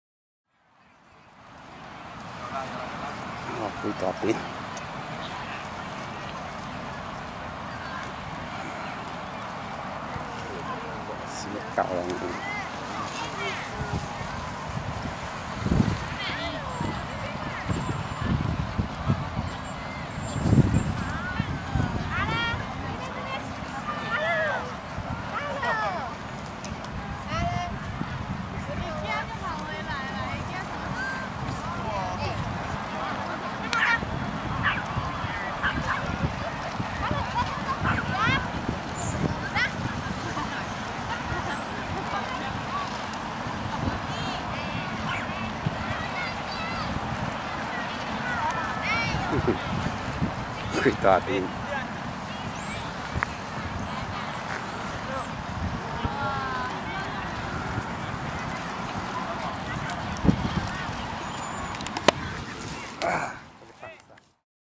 大佳河濱公園溜狗
好多狗，各式各樣，狗比人多! 靠，放屎了! 快去收大便! XD